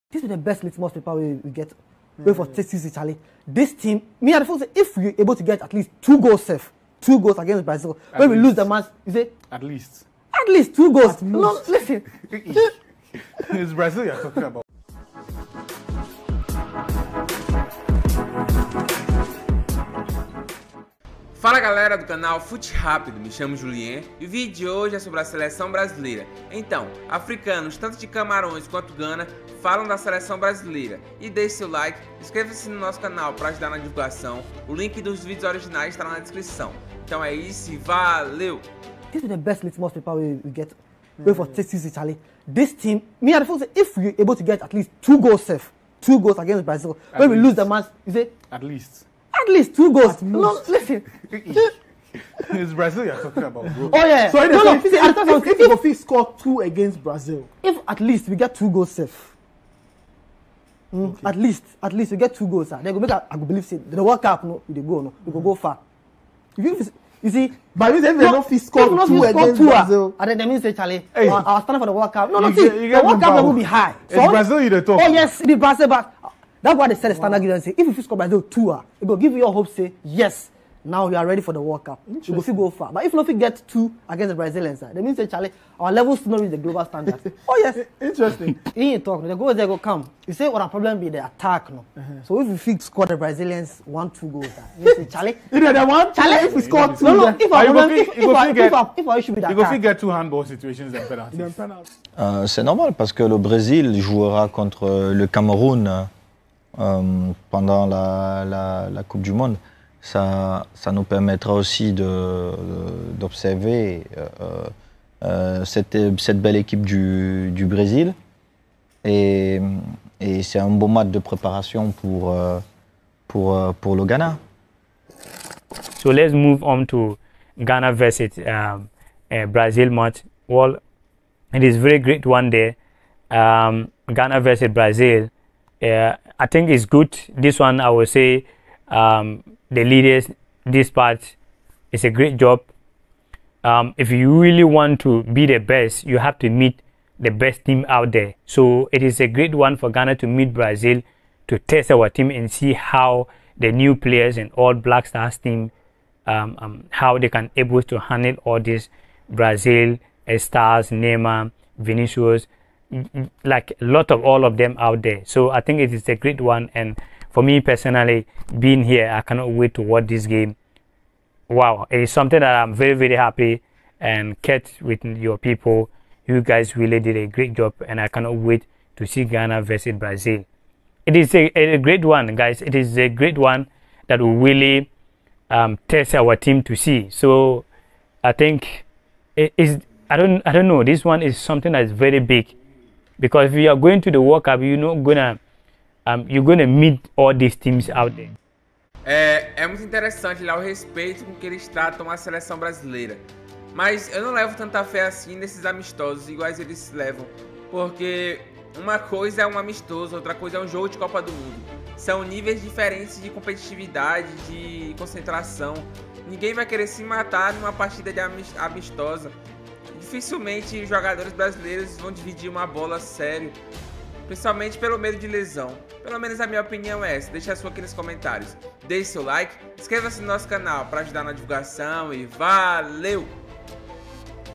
Africanos De Gana E Camarões Falam Do Brasil
A seleção brasileira vem chamando a atenção de todos no mundo pela grande fase de seus jogares!!!

Neymar, Vinicius Júnior, Rodrygo, Pombo, são jogadores que estão vivendo uma fase espetacular!!!

Neste vídeo você verá africanos falando sobre o Brasil e seleção brasileira…